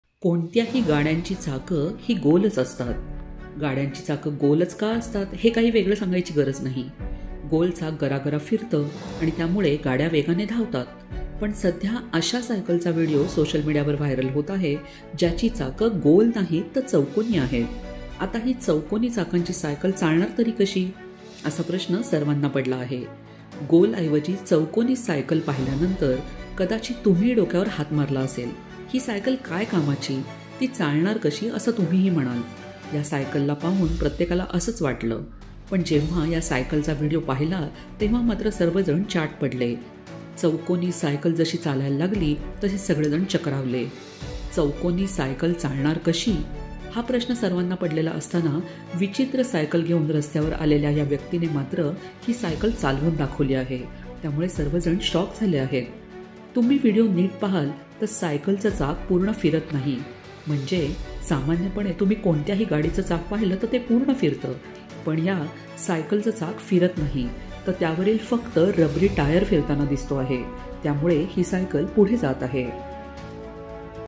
गोलऐवजी चौकोनी चाकांची सायकल; पण चालणार कशी तुम्हीच पाहा VIDEO। M Times Marathi
marathi news thakre
today marathi news
marathi car news xuv400
marathi car news xuv700
xe variant covid marathi news